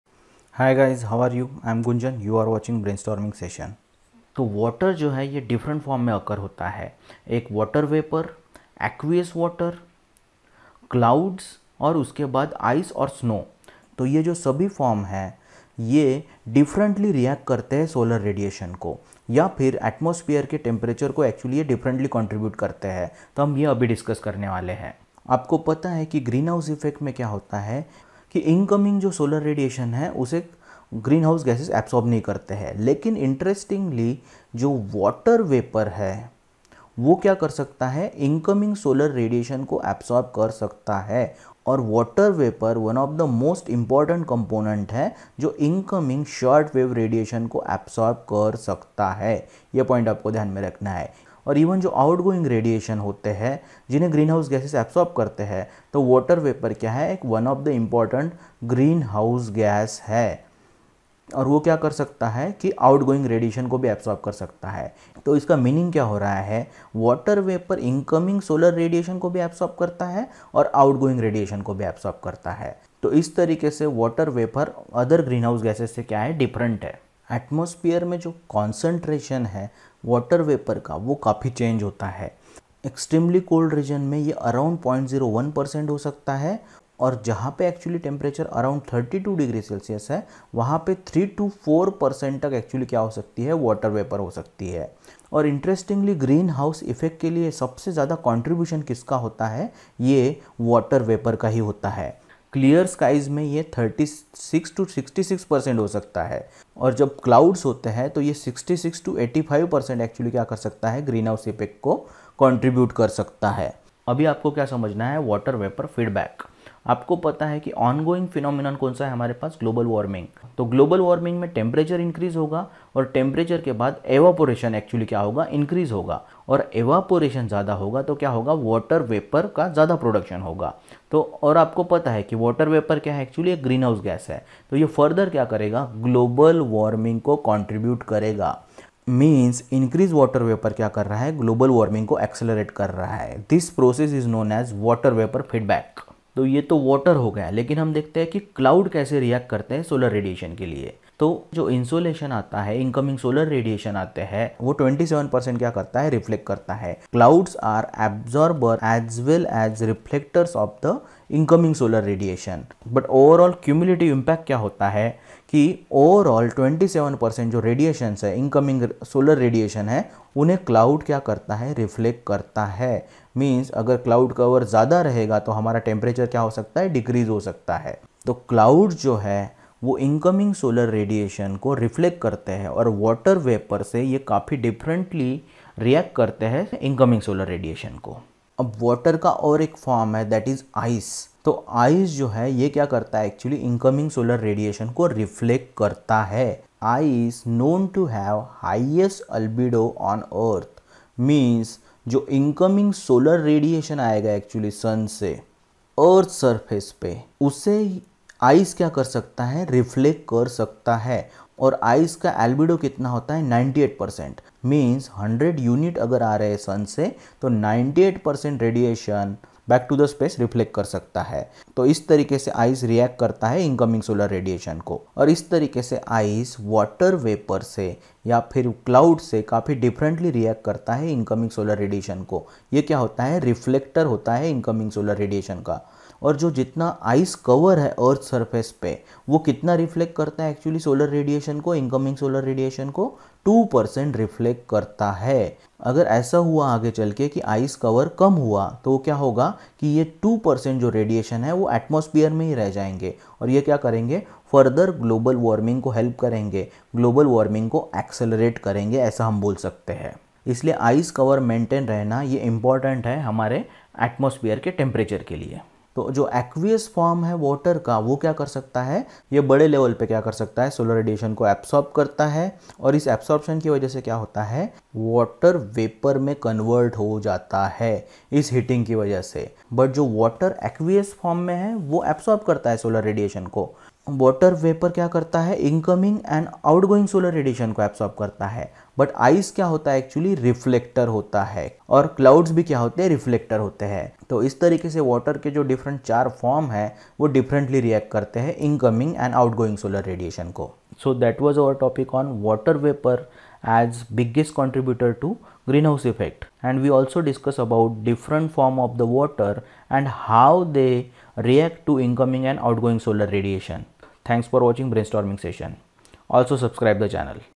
WATER VAPOR As a BIGGEST CONTRIBUTOR TO GREEN HOUSE EFFECT
WATER VAPOR is biggest contributor to green house effect
water occurs in different from such as water , vapor, clouds, snow and react differently to solar radiation.
UPSC environment and ecology